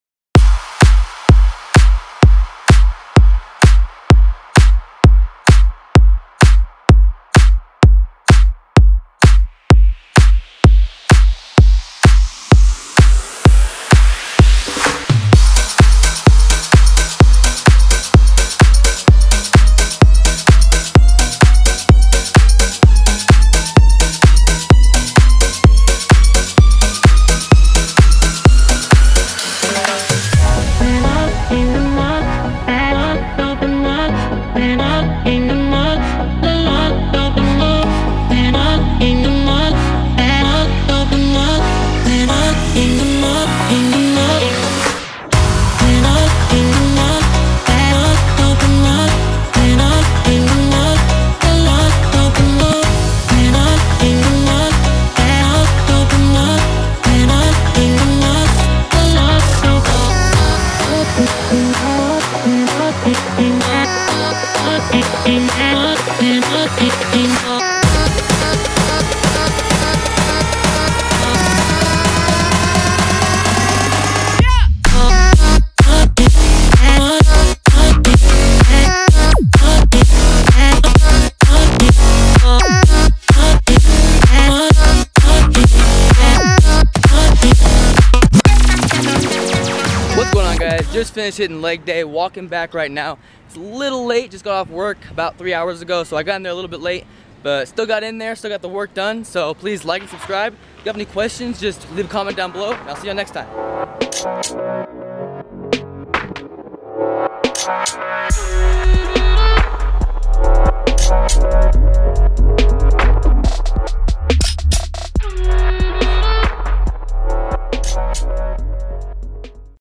Leg Day | Working on Squat Form
What's goin on guys, today I hit an intense leg day really focusing on my squat form.